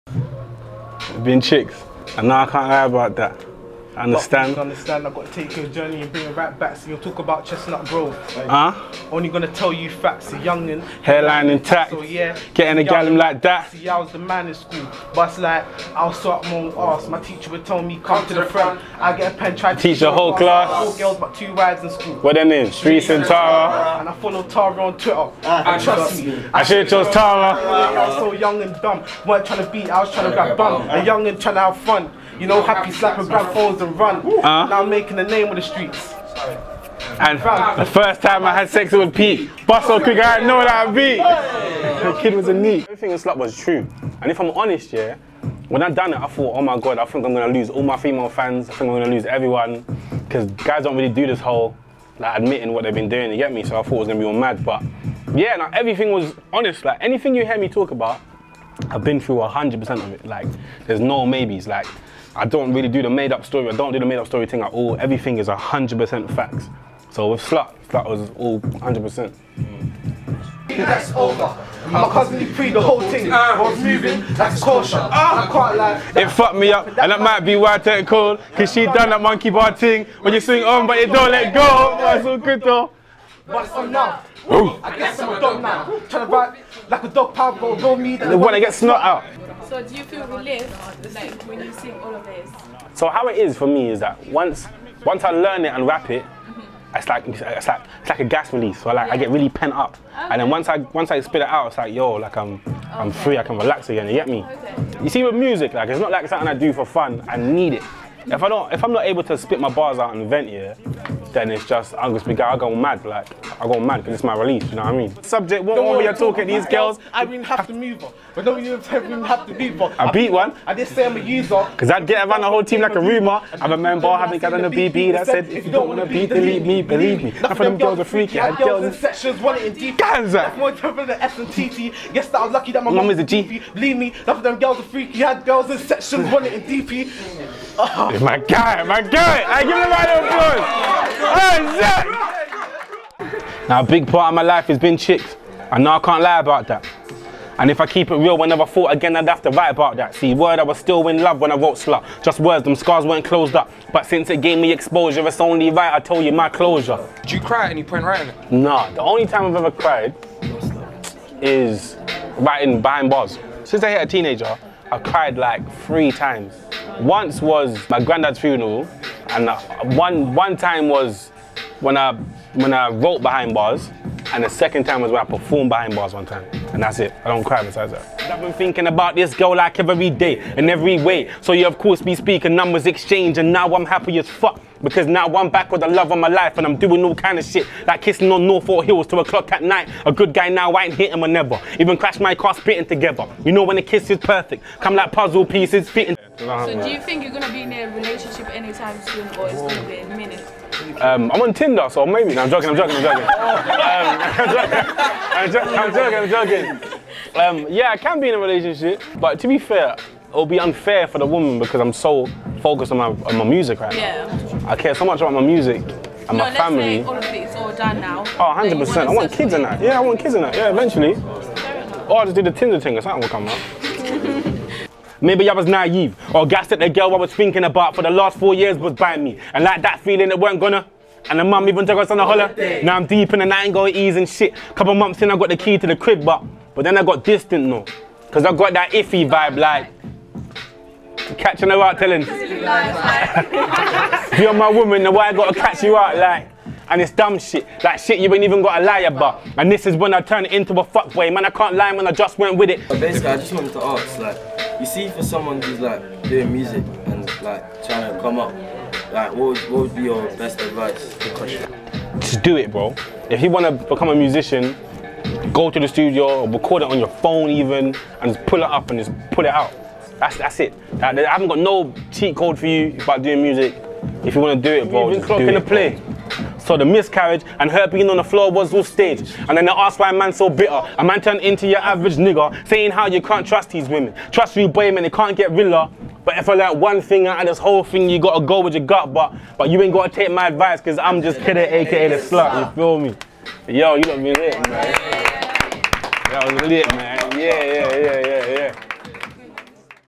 Cadet Freestyle and Interview
Cadet dropped by the Footaylum Croyden store for an in-store performance and Q + A session.

Watch the South London rapper spit bars in front of our trainer wall and talk motivations, crying and Tinder.